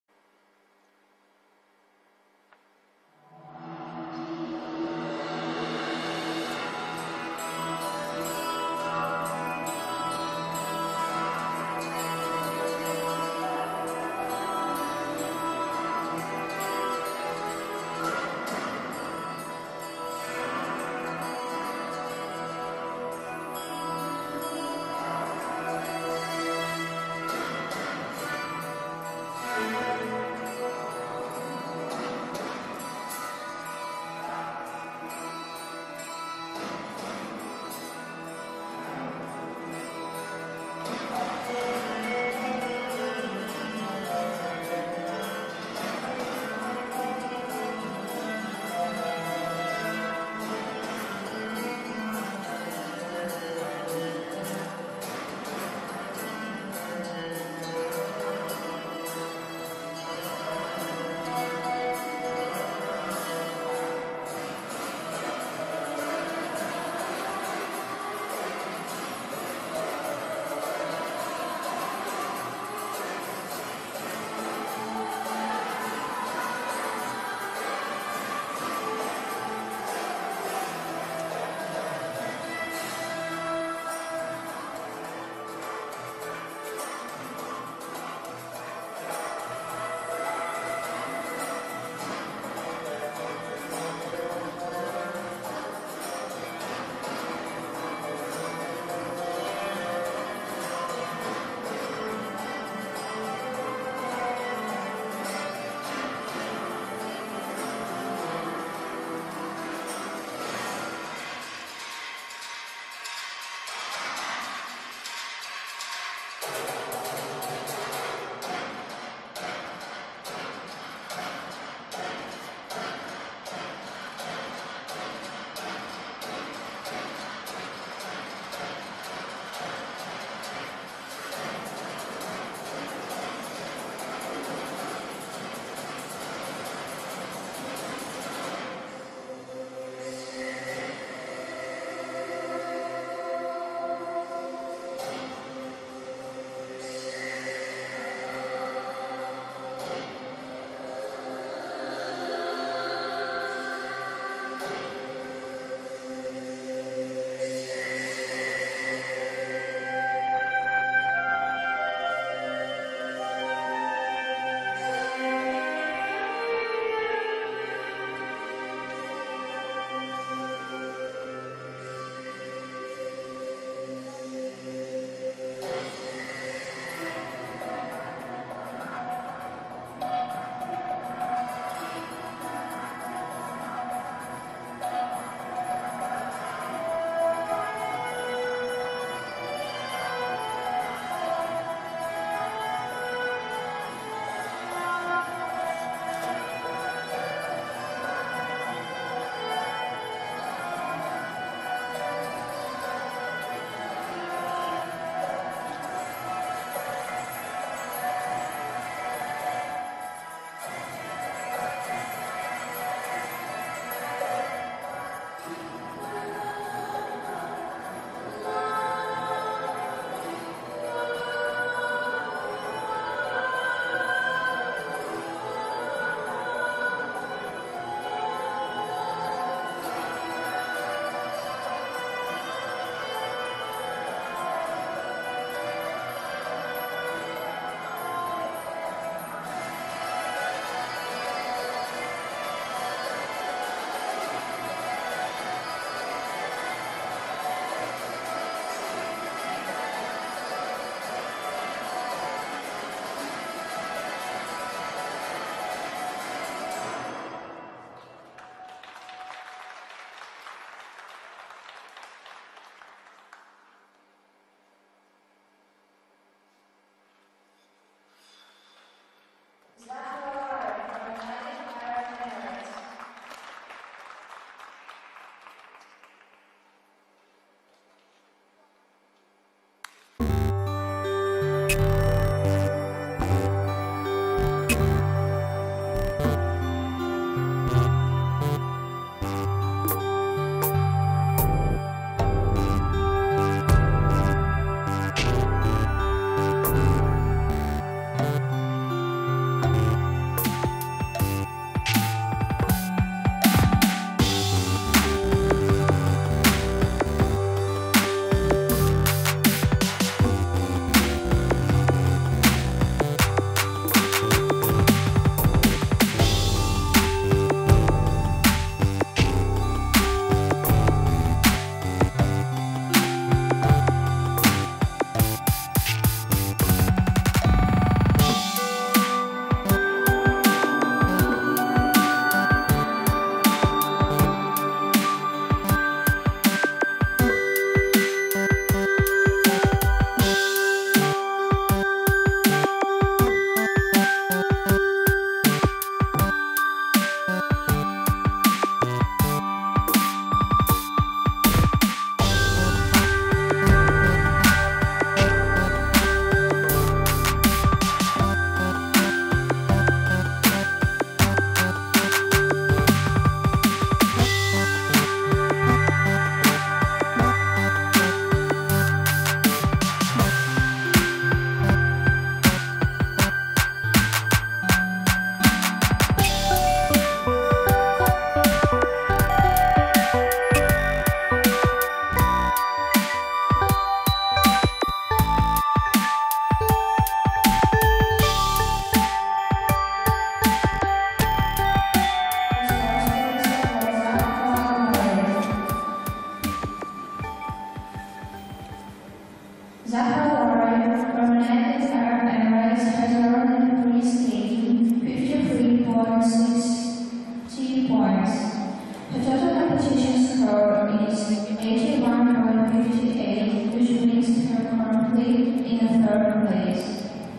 Zahra Lari - Tallinn Trophy FS
Zahra Lari competing her free skate at Tallinn Trophy, 2018